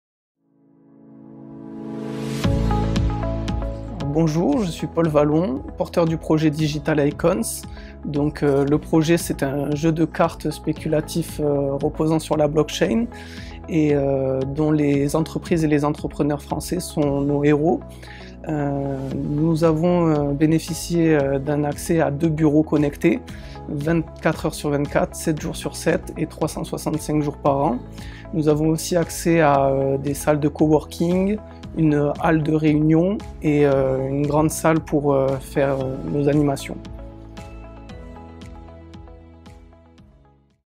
UPVD IN CUBE : présentation du projet DIGITAL ICONS
Projet porté par une équipe composée de Paul WALLON, Romain GARCIA, Tristan GAZULES, Pablo TESTANIER, Charly DAUVEY, Paulin PUNZANO et Nicolas PASCOT.

DIGITAL ICONS est une marketplace de NFT dédiée à la commercialisation, pour les entreprises, d’accès VIP accordant à leur détenteur des prestations de services Premium. Digital Icons entend par là métamorphoser les méthodes de consommation, en permettant aux entreprises de disposer d’un nouveau mode de commercialisation de leurs produits et services.

Ils font partie de la 4ème promotion accompagnée au sein de l'incubateur d'entreprises innovantes de l'Université de Perpignan Via Domitia : UPVD IN CUBE.